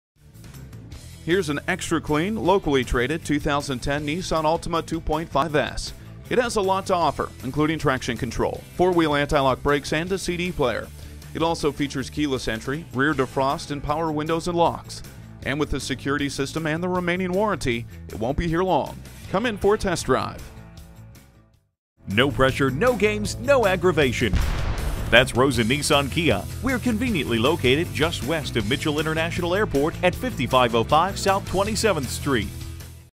2010 Nissan Altima 23940A in Milwaukee Waukesha, WI - SOLD
SOLD - If you are looking for real value on a great used car, Rosen Nissan Kia of Milwaukee invites you to call and make an appointment to view this 2010 Nissan Altima, stock# 23940A. We are conveniently located near Milwaukee Waukesha, WI and known for our great selection, reliability and quality. Call to take a look at this 2010 Nissan Altima today.

5505 South 27th street
Milwaukee Waukesha WI, 53221